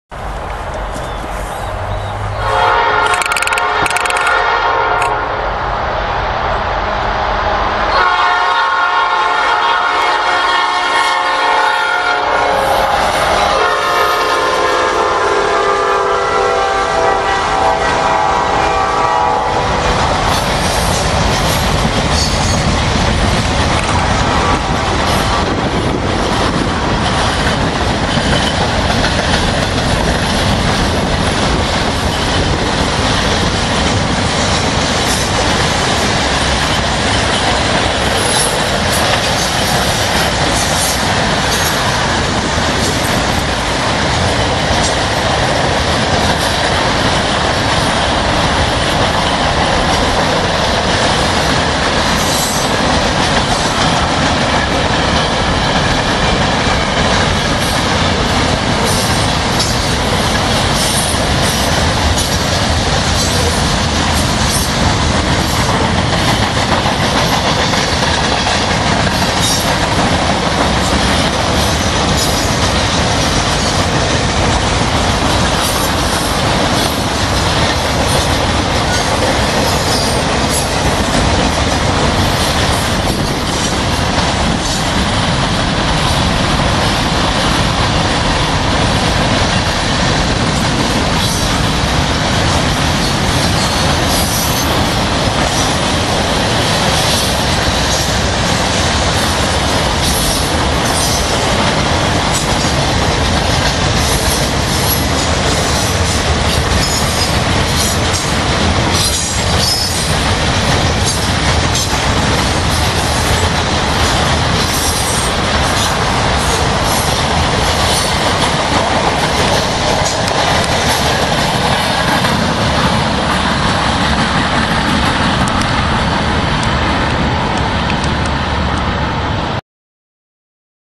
Norfolk Southern 118, Harrisburg NC
Norfolk Southern train 118, North bound through Harrisburg, NC.  3-5-11